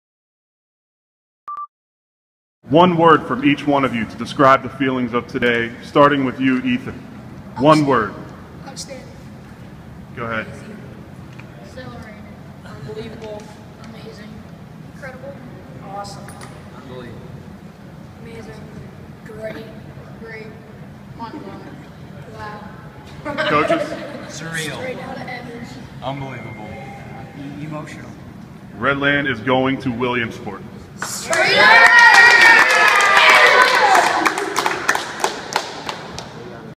Red Landers describe emotion of the day
Each gives a single word to explain the clinching of a Little League World Series berth